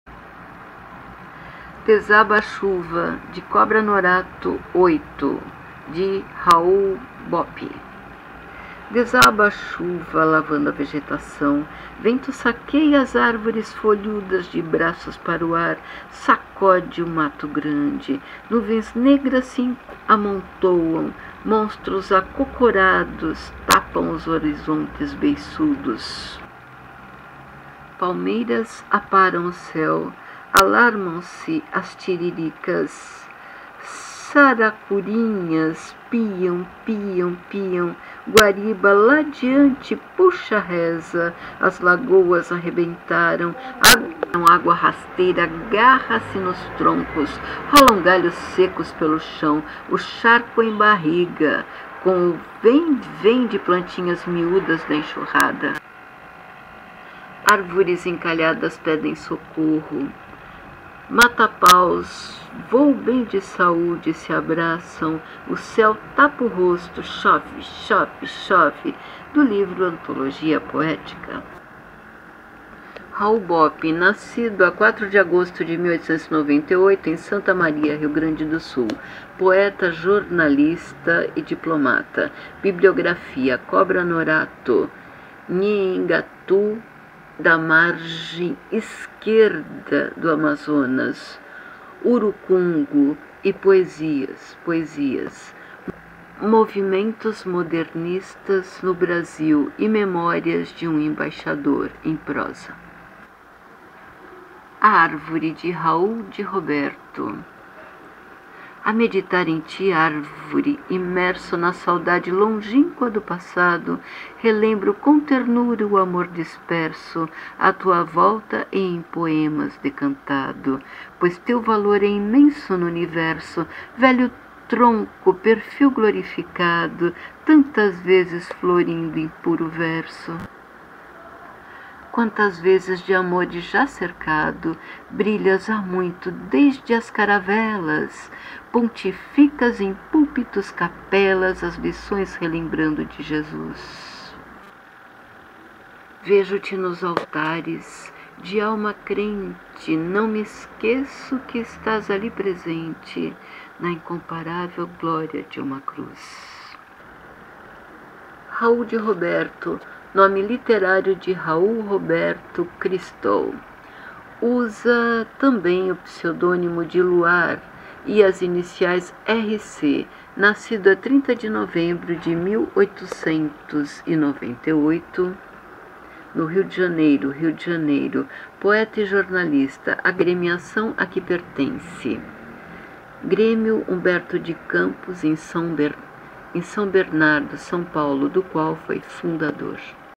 Poesias sobre Plantas 28 - Nova Antologia Brasileira da Árvore - 1974
Livro de Maria Thereza Cavalheiro.
(Não pretendo monetizar os trechos desse livro.)